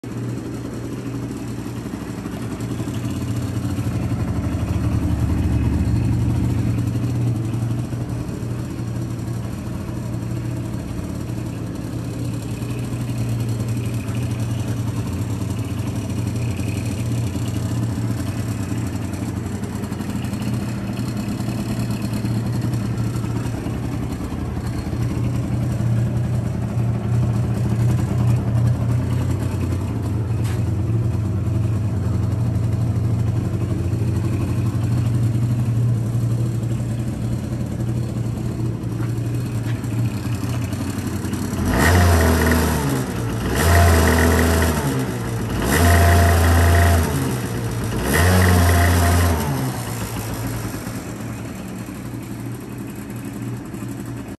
【販売車両紹介】ホンダ ジャイロキャノピー TA02 佐賀武雄 Garage-J イオ
ホンダ　ジャイロキャノピー　TA02
2007年モデル　走行距離38560km

初めまして、Garge-J イオです。
現車確認、下見の場合は事前にご連絡をお願い致します。

Nice to meet you, I'm Garge-J Io.

〒849-2201
638P+FR 武雄市、佐賀県